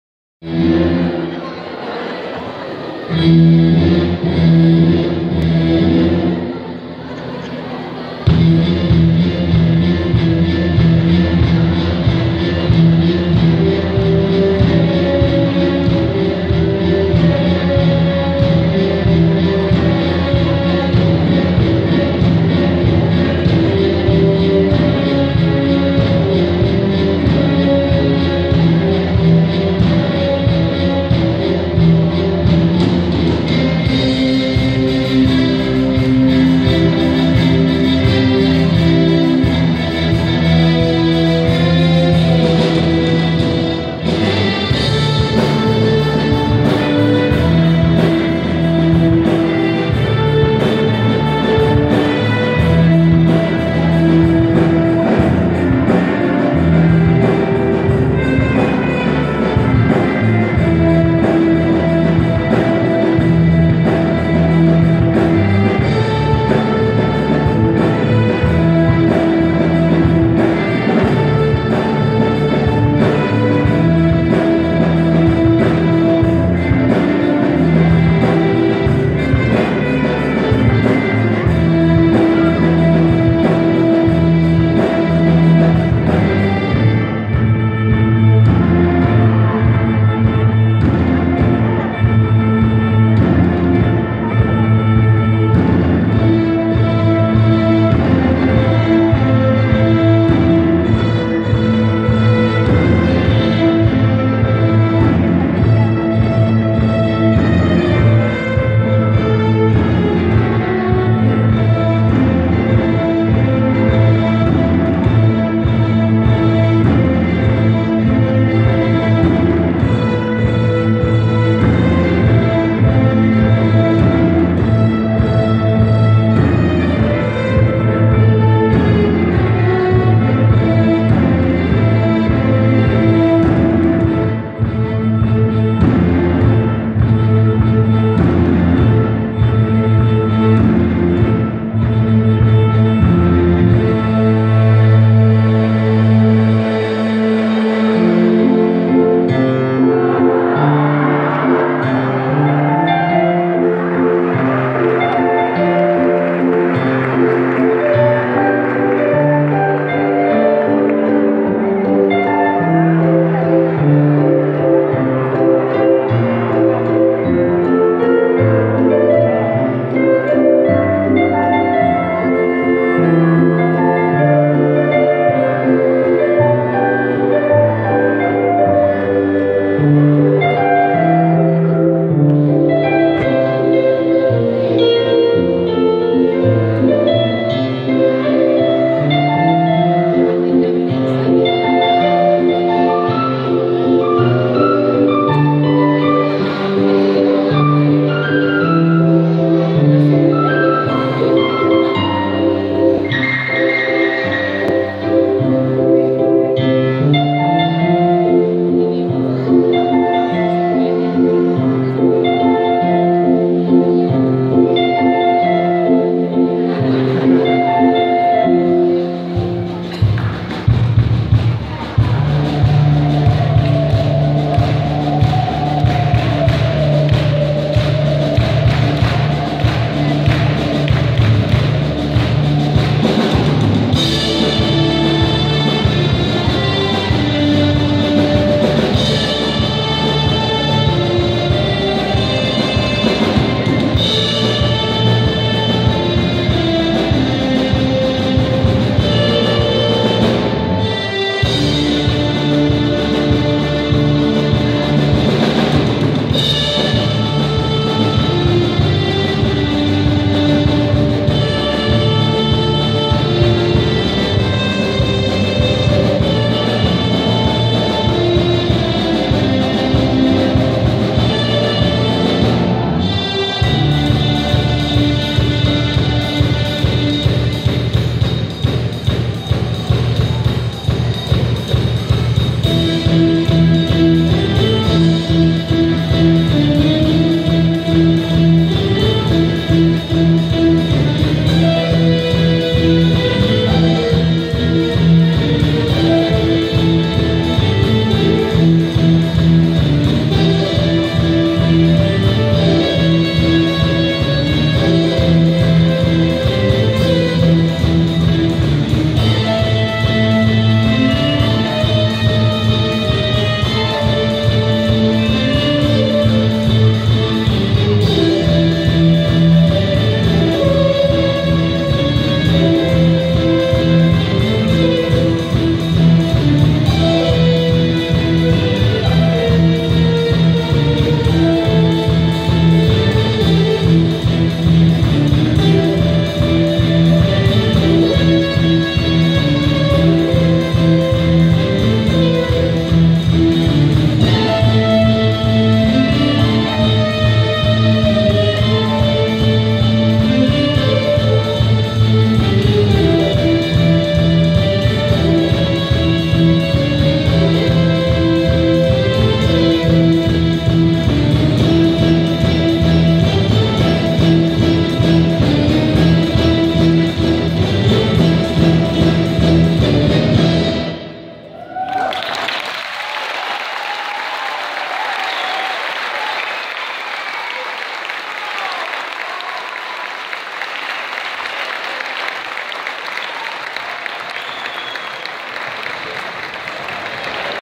Mix de canciones de peliculas
Presentación taller de Música instrumental ed. básica ...colegio valle de colchagua 2016